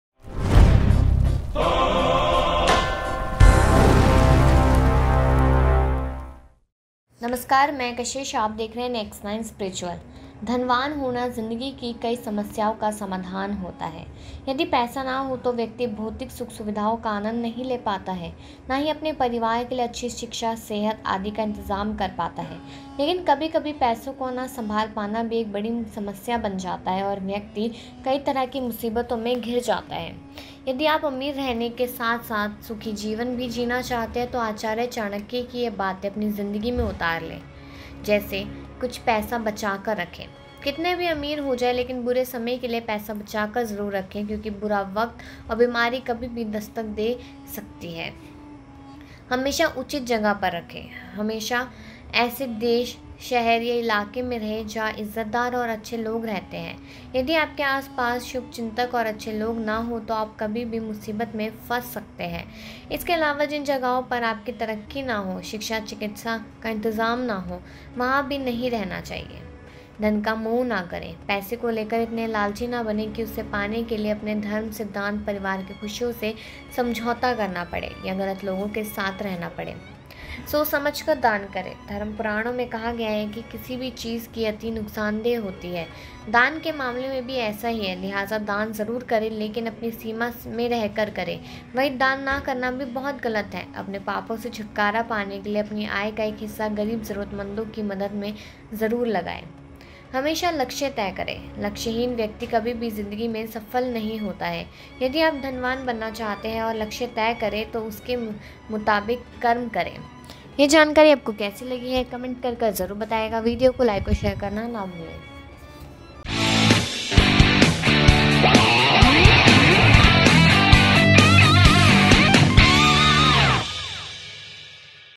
धनवान बनना चाहते हैं तो जरूर जान लें चाणक्य नीति की ये 5 बातें, हमेशा रहेंगे सुखी
धनवान होना जिंदगी की कई समस्यारओं का समाधान होता है. यदि पैसा न हो तो व्यिक्ति भौतिक सुख-सुविधाओं का आनंद नहीं ले पाता है, ना ही अपने परिवार के लिए अच्छीि शिक्षा-सेहत आदि का इंतजाम कर पाता है. लेकिन कभी-कभी पैसों को न संभाल पाना भी एक बड़ी समस्याप बन जाता है और व्यछक्ति कई तरह की मुसीबतों में घिर जाता है. यदि आप अमीर रहने के साथ-साथ सुखी जीवन भी जीना चाहते हैं, तो आचार्य चाणक्यज की ये बातें अपनी जिंदगी में उतार लें.